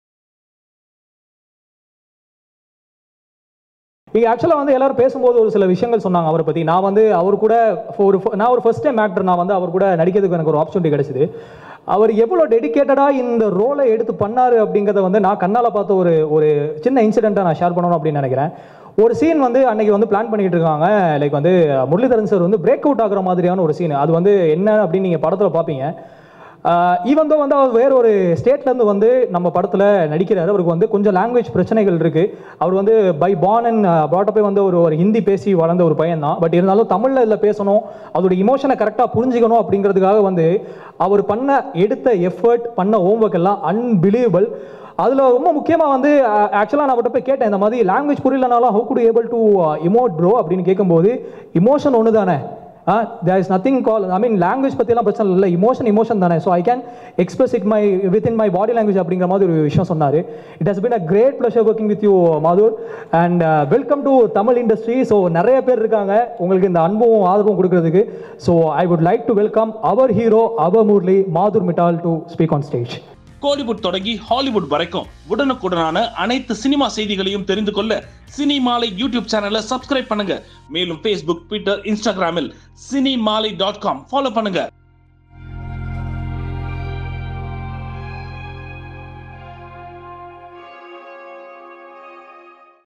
800 The Movie Press Meet Tamil | Muttiah Muralitharan | Madhurr Mittal | Mahima Nambiar
800TheMovie Starring Madhurr Mittal as Muralidharan.

Movie - 800
Starring - Madhurr Mittal
Directed by MS Sripathy
Produced by Movie Train Motion Picture & Vivek Rangachari
Music Composer: Ghibran
Cinematographer: RD Rajasekar ISC
Editor: Praveen KL
Written by: MS Sripathy & Shehan Karunatilaka
Production Designer: Videsh
Costume Designers: Poorthi Pravin & Vipin PR
Line Producer: Kandhan Pitchumani
VFX Supervisor: Jithendra Vijaykumar
News portal that covers Tamil Cinema and other Tamil Entertainment